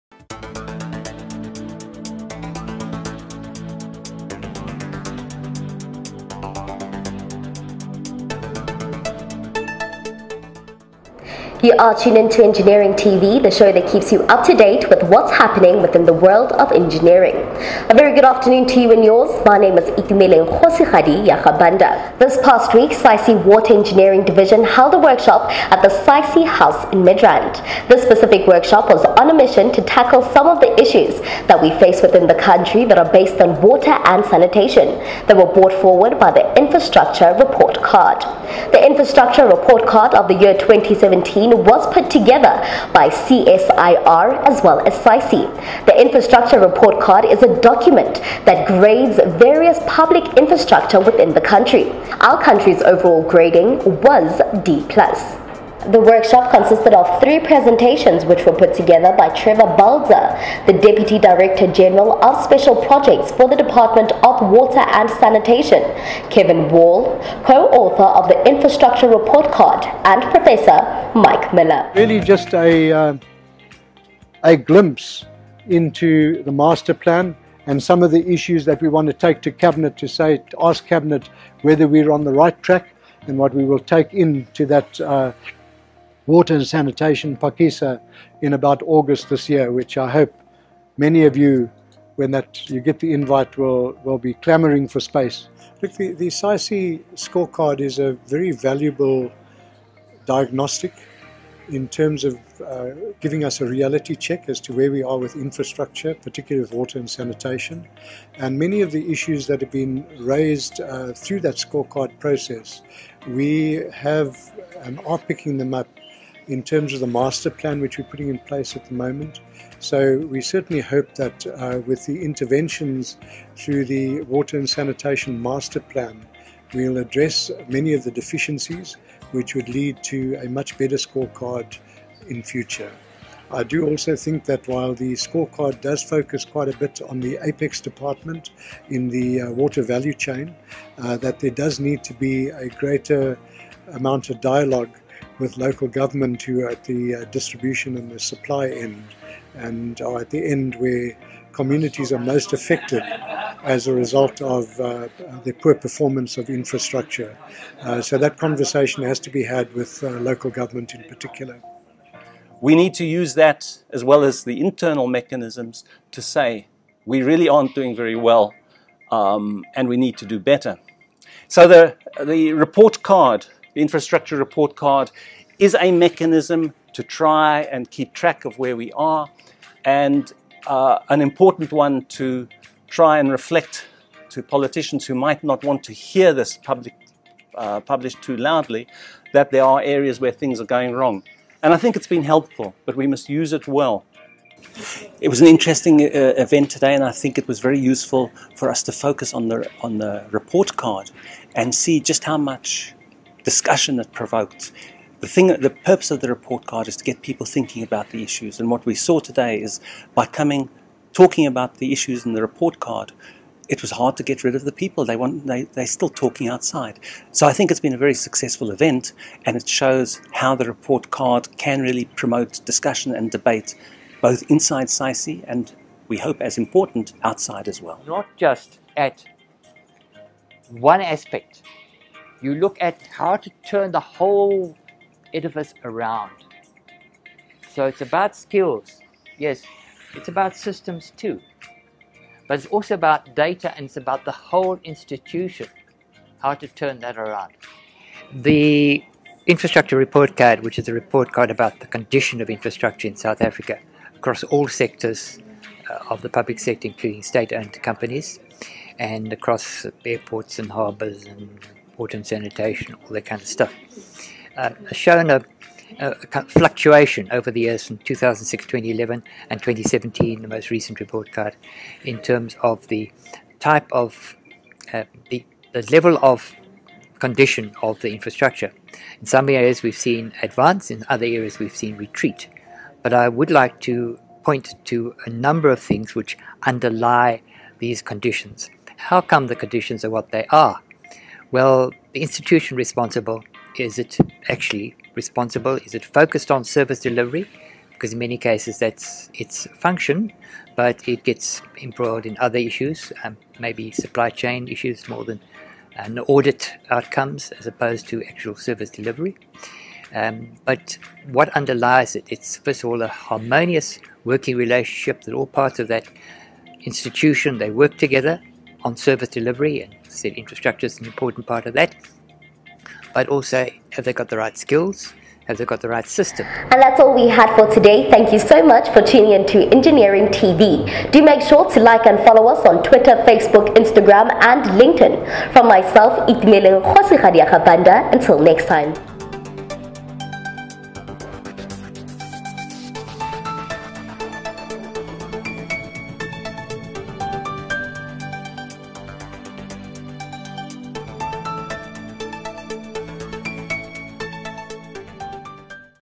Engineering TV SAICE Water Engineering Division Workshop 2018
SAICE Water Engineering Division held a workshop addressing water and sanitation issues brought forth by the IRC 2017.